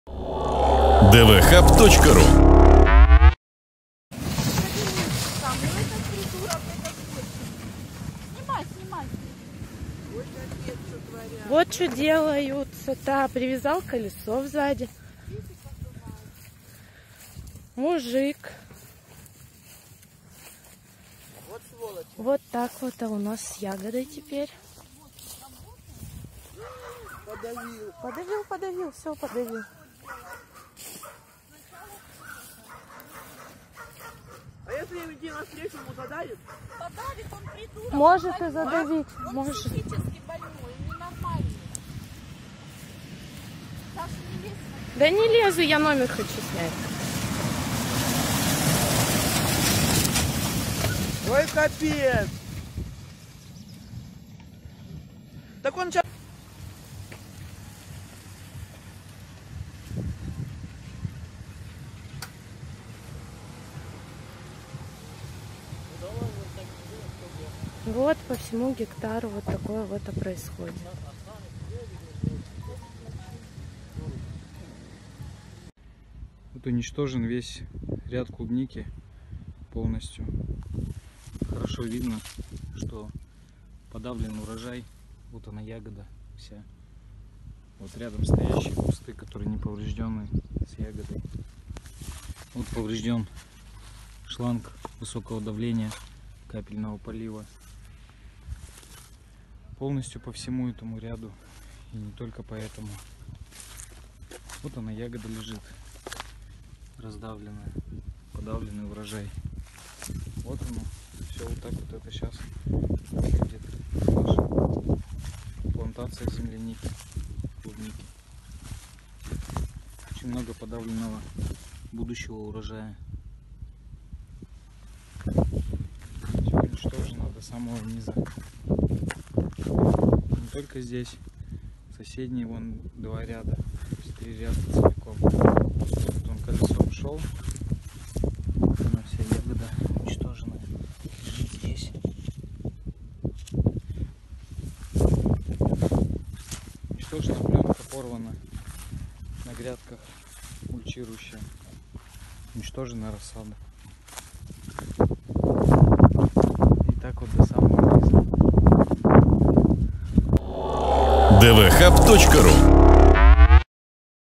Представитель губернатора уничтожил фермерскую клубнику на дальневосточном гектаре
Уполномоченный представитель губернатора Хабаровского края в Комсомольске-на-Амуре решил разобраться с конкурентами в стиле 90-х. Мужчина привязал к своему внедорожнику колесо от трактора и проехал по урожаю клубники, принадлежавшей соседу.